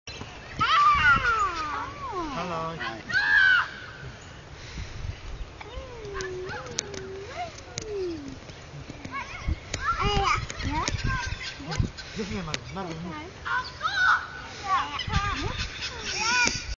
Seesaw with Oma, Oct 2010
Trying out the seesaw for the second time, this time with Oma, and Daddy assisting.